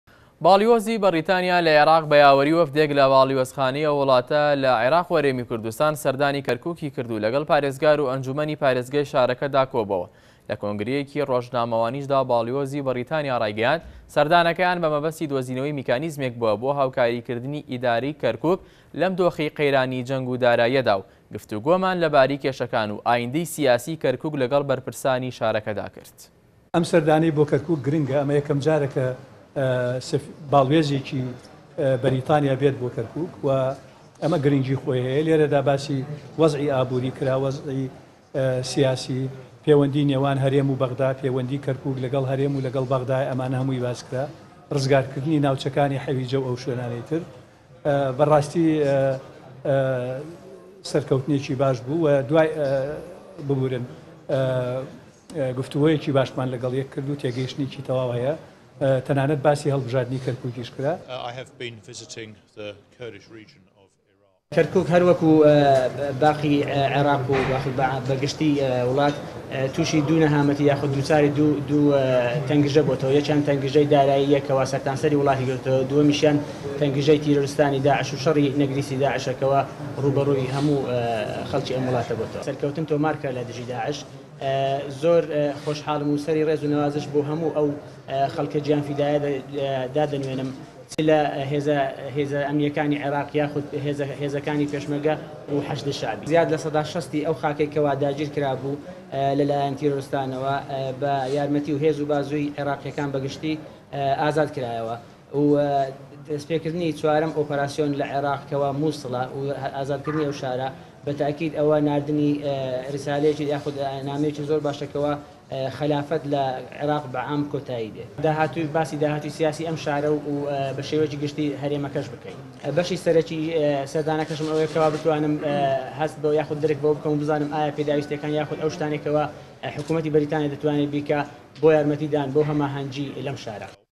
باڵیۆزی‌ به‌ریتانیا له‌ عێراق له‌گه‌ڵ پارێزگار‌و ئه‌نجومه‌ی‌ پارێزگای كه‌ركوك كۆده‌بێته‌وه‌
باڵیۆزی‌ به‌ریتانیا له‌ عێراق به‌ یاوه‌ریی‌ وه‌فدێك له‌ باڵیۆزخانه‌ی‌ ئه‌و وڵاته‌ له‌ عێراق‌و هه‌رێمی‌ كوردستان, سه‌ردانی‌ كه‌ركوكی‌ كرد‌و له‌گه‌ڵ‌ پارێزگار‌و ئه‌نجومه‌نی‌ پارێزگای‌ شاره‌كه‌دا كۆبوه‌وه‌.

له‌ كۆنگره‌یه‌كی‌ رۆژنامه‌وانیشدا, باڵیۆزی‌ به‌ریتانیا ڕایگه‌یاند: سه‌ردانه‌كه‌یان به‌مه‌به‌ستی‌ دۆزینه‌وه‌ی‌ میكانیزمێك بوه‌ بۆ هاوكاریكردنی‌ ئیداره‌ی‌ كه‌ركوك له‌م دۆخی‌ قه‌یرانی‌ جه‌نگ‌و داراییه‌دا‌و گفتوگۆمان له‌باره‌ی‌ كێشه‌كان‌و ئاینده‌ی‌ سیاسی‌ كه‌ركوك له‌گه‌ڵ به‌رپرسانی‌ شاره‌كه‌دا كرد.